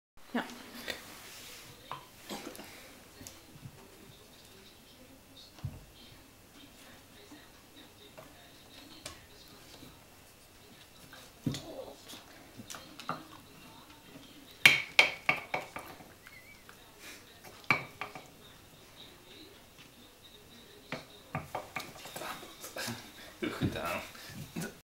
Intelligentiespeelgoed hond - trekblok - Dot lost het probleem op in 15''
Leuk filmpje waarin Dotje (donker keeshondje) het probleem oplost in 15''.